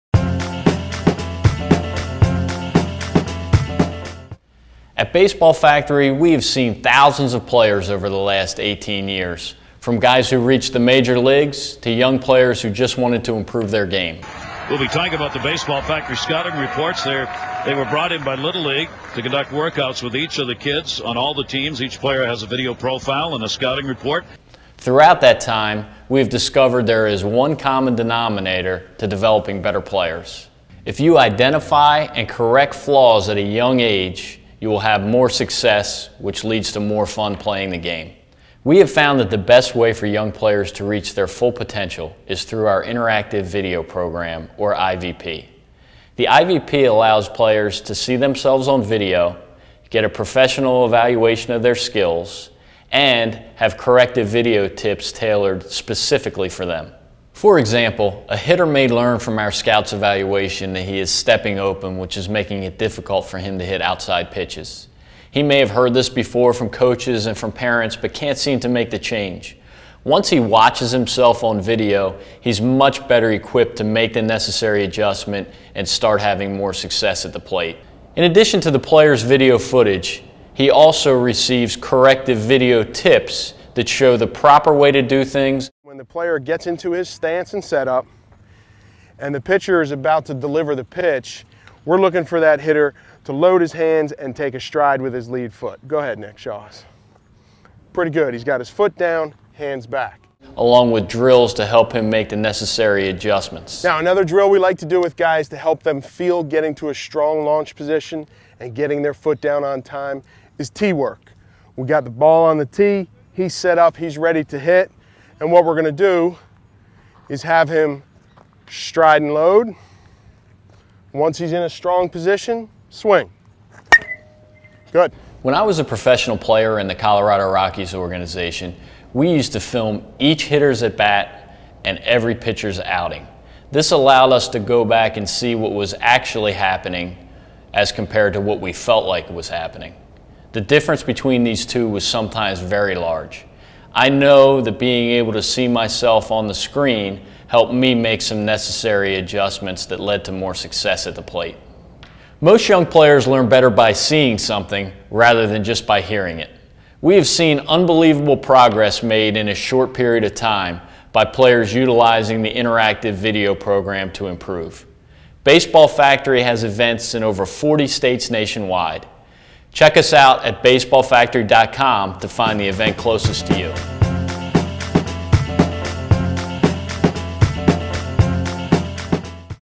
Baseball Factory Interactive Video Program for Youth
The nation's premier YOUTH DEVELOPMENT program. Baseball Factory's IVP (Interactive Video Program) is for players ages 12-14. More than just a one day Under Armour Tryout, filming, & training session. It's an opportunity for kids who love the game to be EVALUATED, learn more, improve, & HAVE FUN doing it. Plus, find out the importance of video as a learning tool.